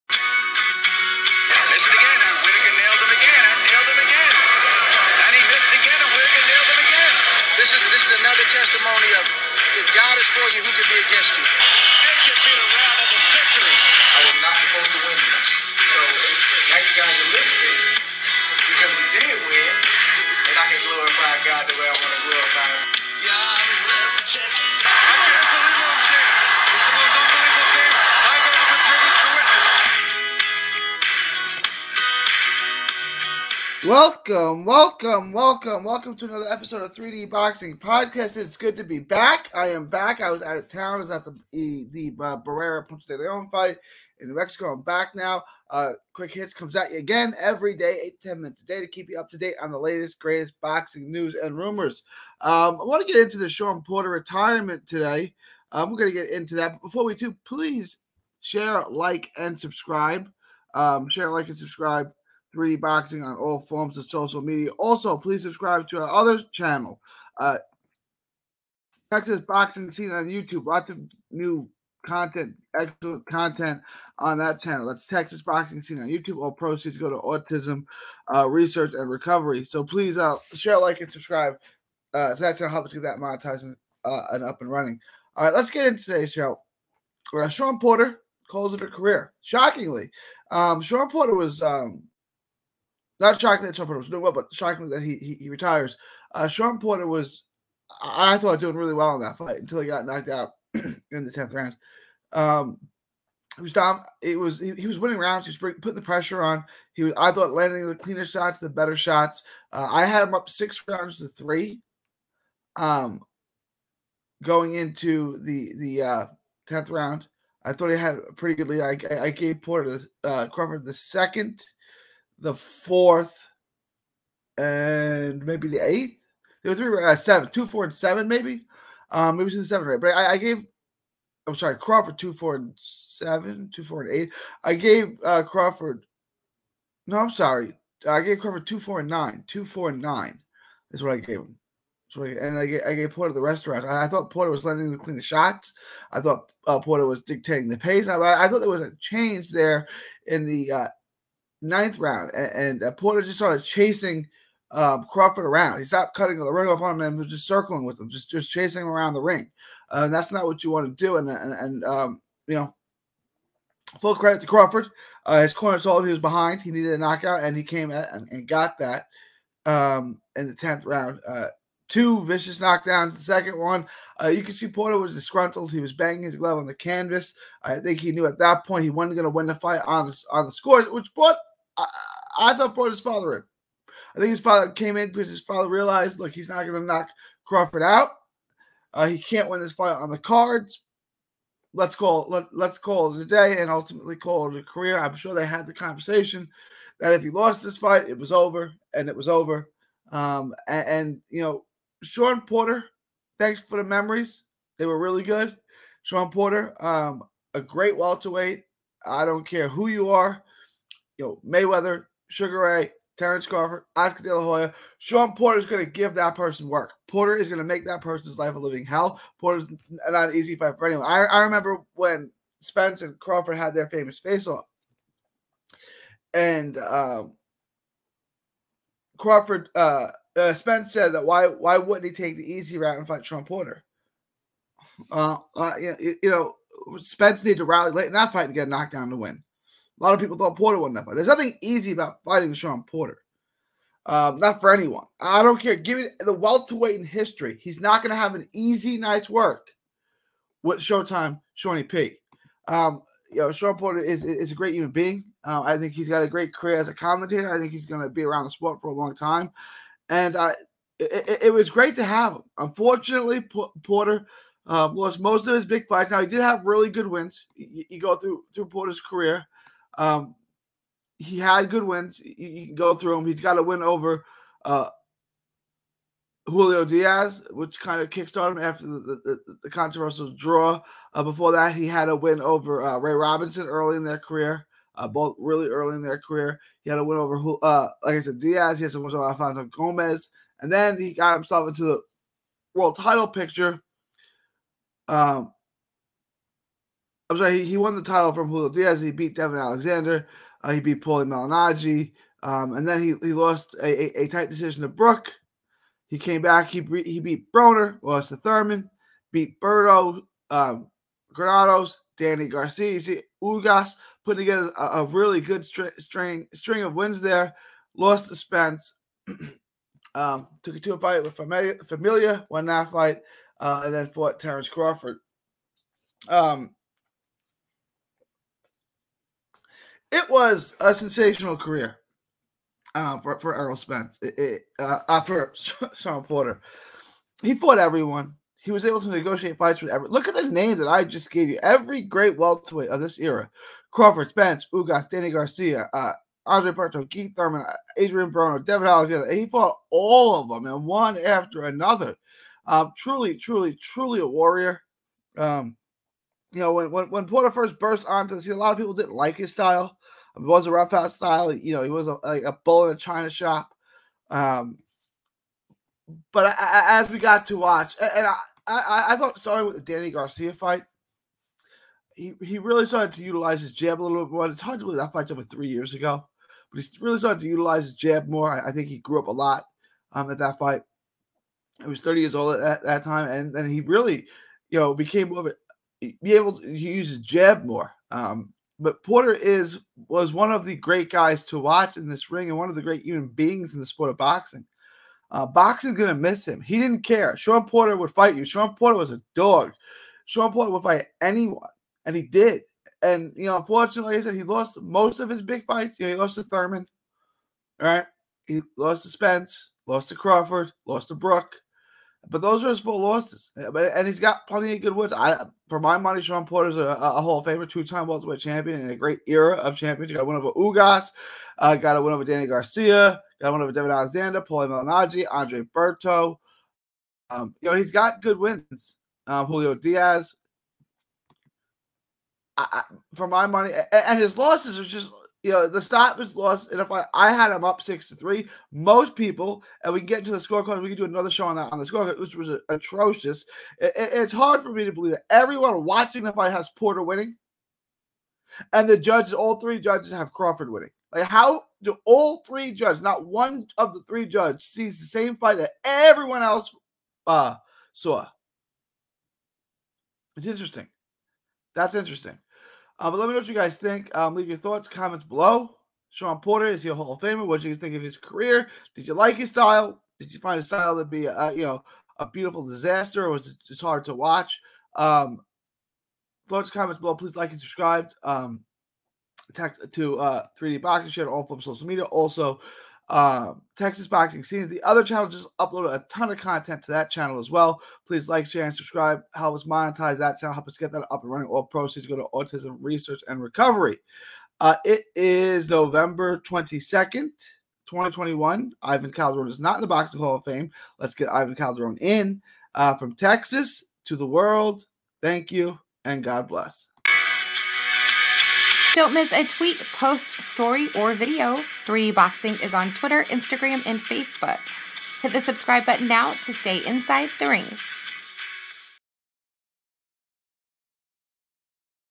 Quick Hits: Shawn Porter Retires!!
Two-Time welterweight champ Shawn Porter announces retirement. Boxing fans owe him a debt of gratitude. Porter's next stop should be the International Boxing Hall of Fame